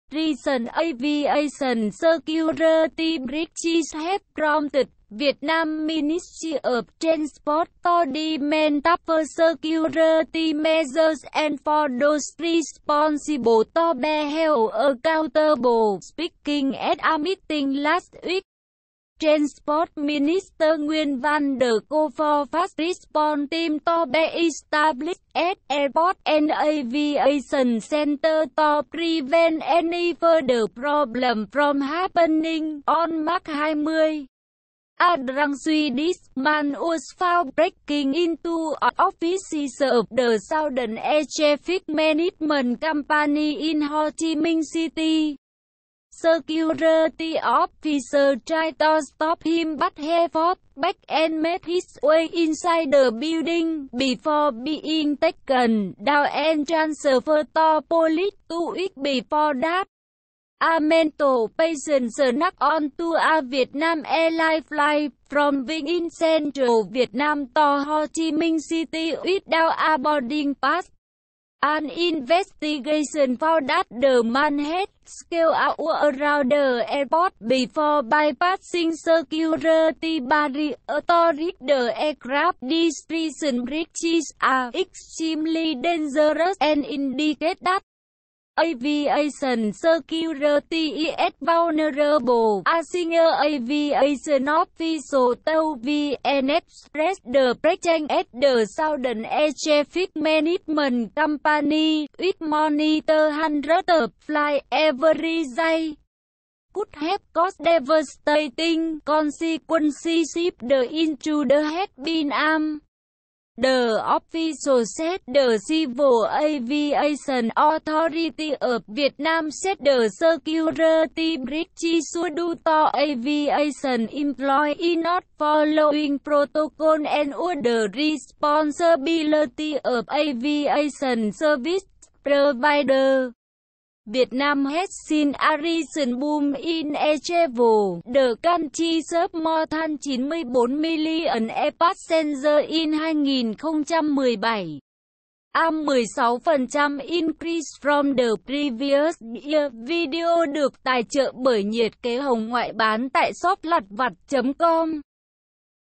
Vietnamese minister calls for tighter aviation security after series of scares
Recent aviation security breaches have prompted Vietnam's Ministry of Transport to demand tougher security measures and for those responsible to be held accountable.
Speaking at a meeting last week, Transport Minister Nguyen Van The called for fast response teams to be established at airports and aviation centers to prevent any further problems from happening.
On March 20, a drunk Swedish man was found breaking into offices of the Southern Air Traffic Management Company in Ho Chi Minh City. Security officers tried to stop him but he fought back and made his way inside the building, before being taken down and transferred to police.
Two weeks before that, a mental patient snuck onto a Vietnam Airlines flight from Vinh in central Vietnam to Ho Chi Minh City without a boarding pass. An investigation found that the man had scaled a wall around the airport before bypassing security barriers to reach the aircraft.
These recent breaches are "extremely dangerous" and indicate that aviation security is vulnerable, a senior aviation official told VnExpress.
The break-in at the Southern Air Traffic Management Company, which monitors hundreds of flights every day, could have caused "devastating" consequences if the intruder had been armed, the official said.
The Civil Aviation Authority of Vietnam said the security breaches were due to aviation employees not following protocols and were the responsibility of aviation service providers.
Vietnam has seen a recent boom in air travel. The country served more than 94 million air passengers in 2017, a 16 percent increase from the previous year.

👉 Hundreds left dazed and confused after apartment fire kills 13 in Ho Chi Minh City h